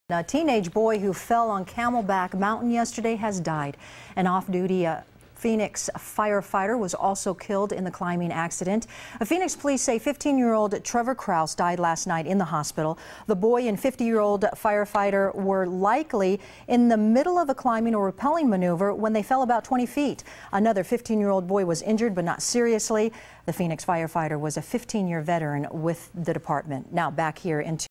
Teen dies in climbing accident
A teenage boy who fell on Camelback Mountain yesterday has died.